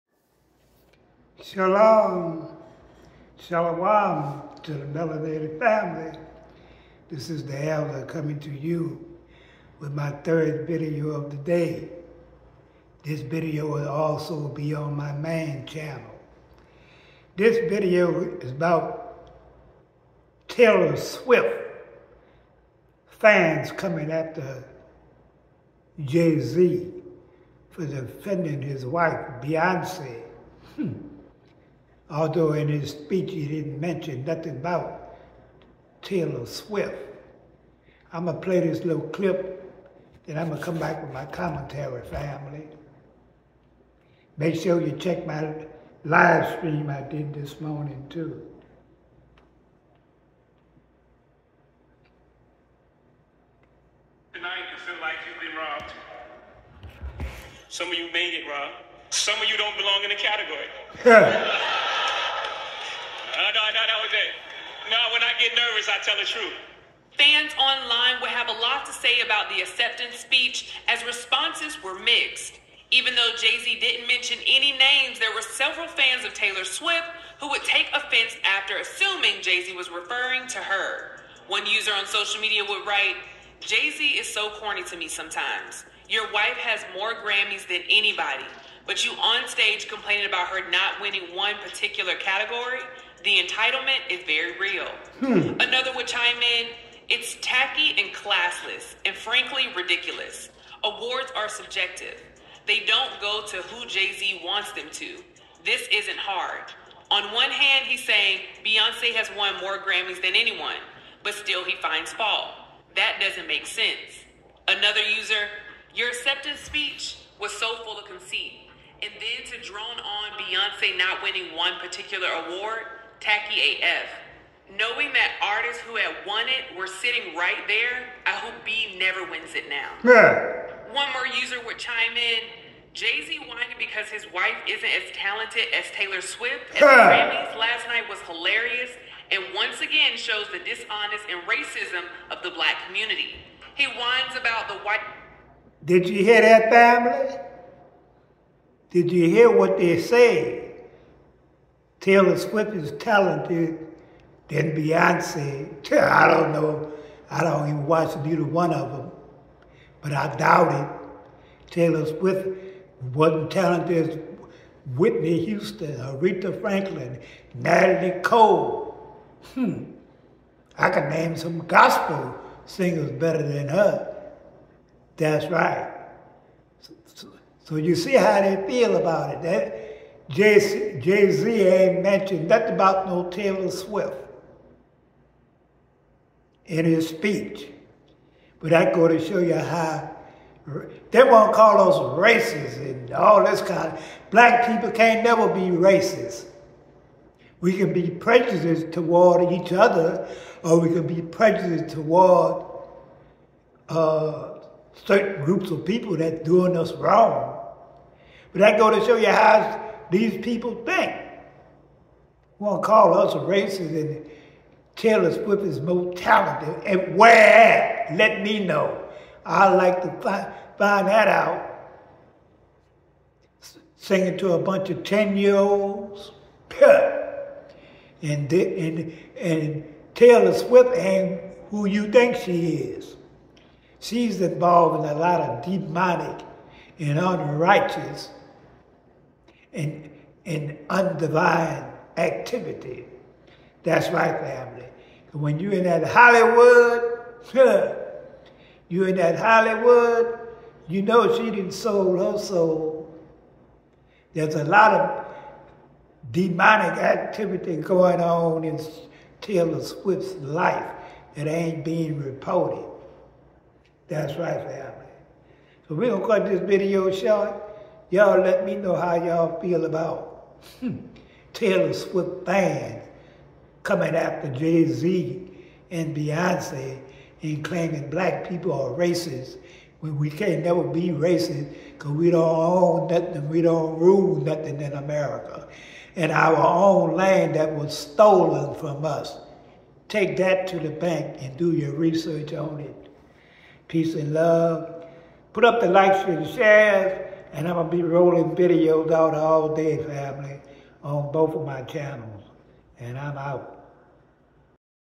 Taylor Swift fans comes after Jay Z for defending his wife beyoncé ?
commentary and content on Taylor Swift fans coming after hip hop star.